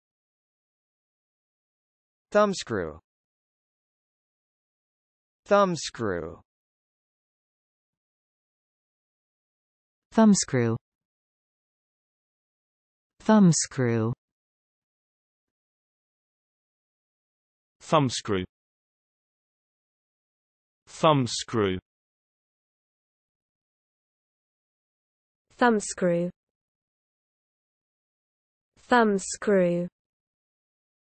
How to pronounce thumbscrew
Learn how to say Thumbscrew in English correctly with TextToSpeech.io free pronunciation tutorials. American & British English pronunciation of male & female voices provided in videos. Check them out!

    Thumbscrew definitions:
    (n): screw designed to be turned with the thumb and fingers
    (n): instrument of torture that crushes the thumb